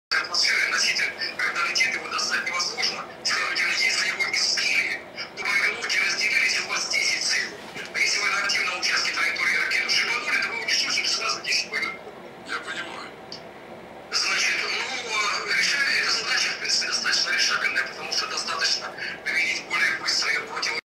Глобус Mova Globe d12 с политической картой мира (бежевый)
Данное видео демонстрирует то, как работает Глобус Mova Globe d12 с политической картой мира (бежевый). Более детально ознакомиться и приобрести данный товар можно по ссылке ниже